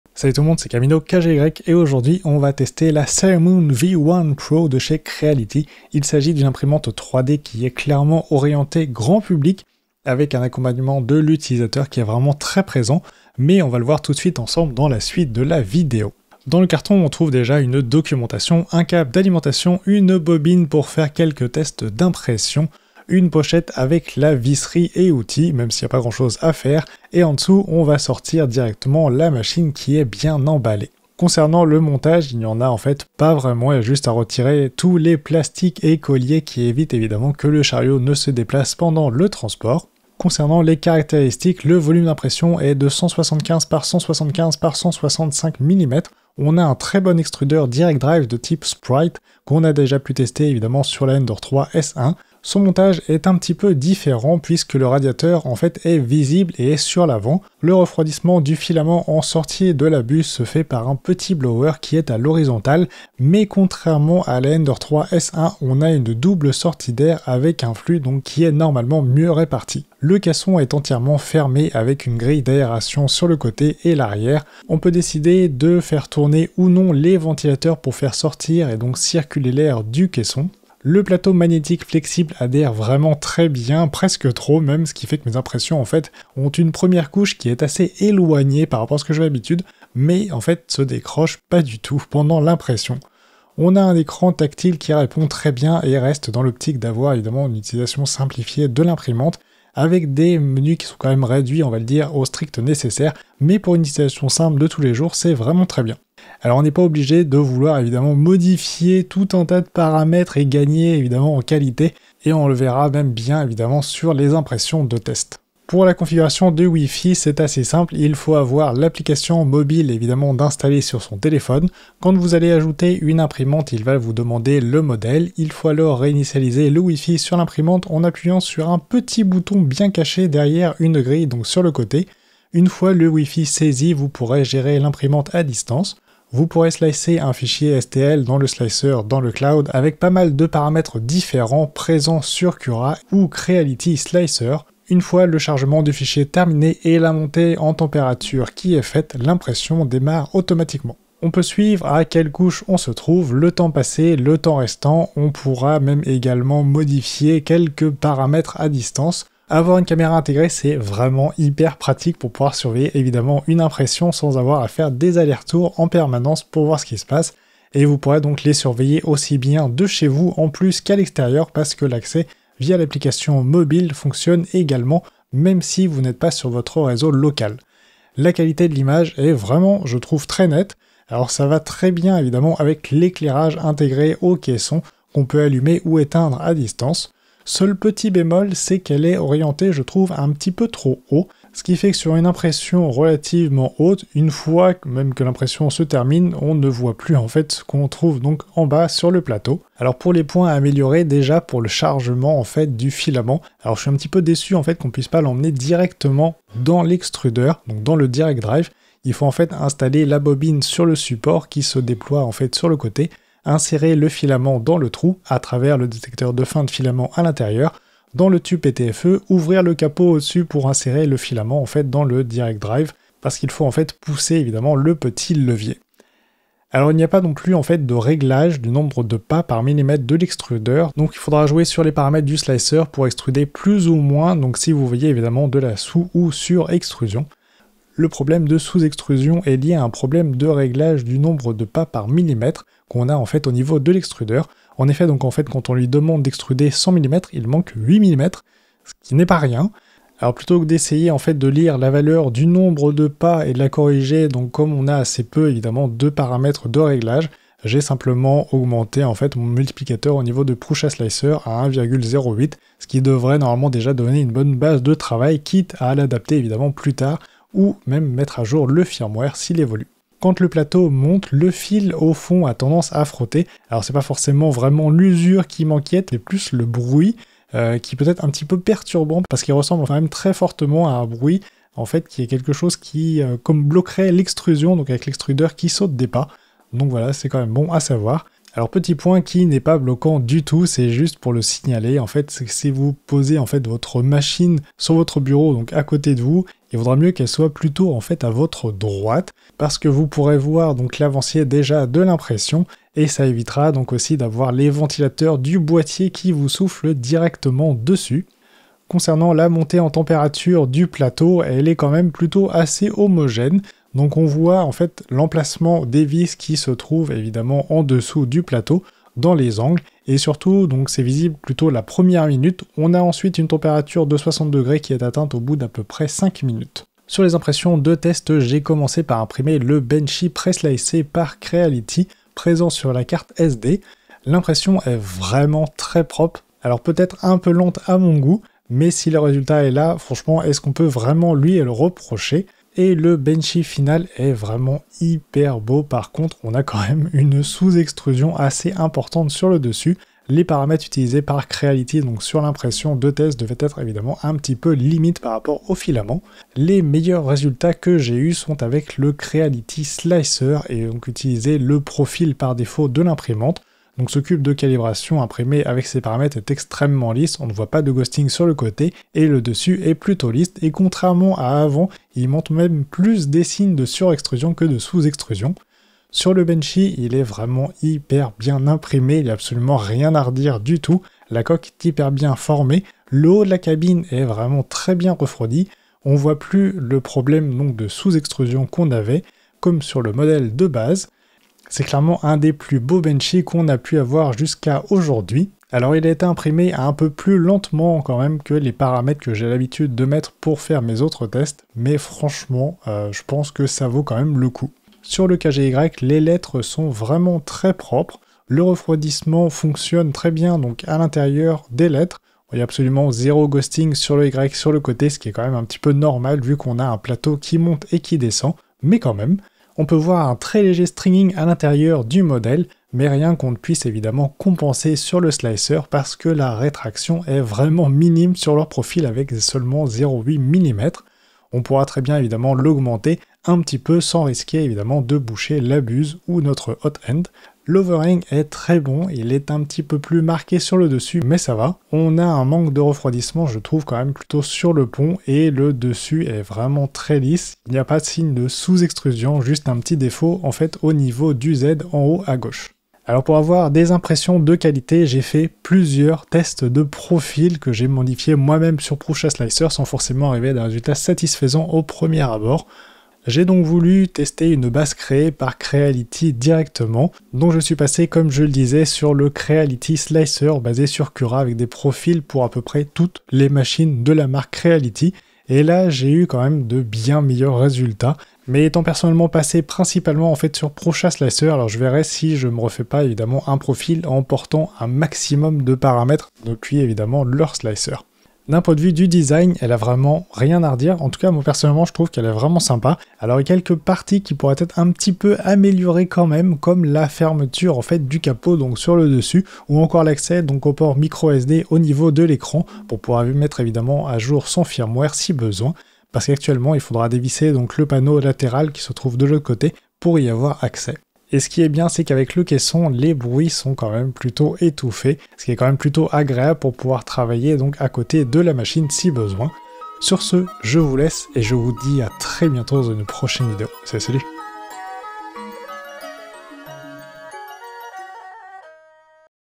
Sermoon V1 Pro Creality - Imprimante 3D fermée grand public
Dans cette vidéo, on va tester Sermoon V1 Pro de chez Creality. Une imprimante 3D fermée qui est orientée grand public.

🖖 LES RESEAUX

📺 DANS LA VIDEO

🛠️ CE QUE J'UTILISE